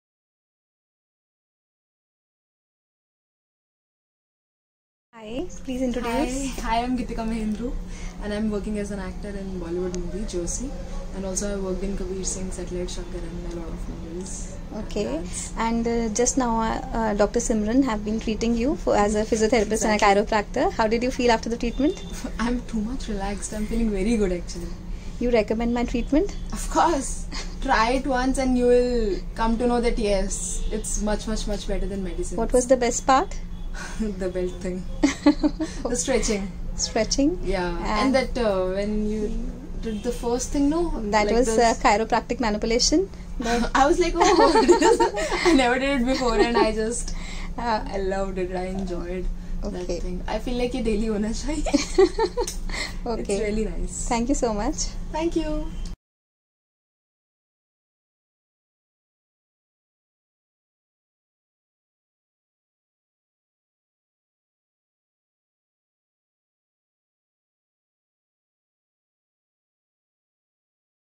Review from Bollywood Actress "Geetika Mahendru"
Have a check on review from famous Bollywood actress after treatment from Dr Simranjeet Kaur

Thanks credit

Sukhija Physiotherapy Clinic Scf33 phase10 Mohali Punjab India

Musician: @iksonofficial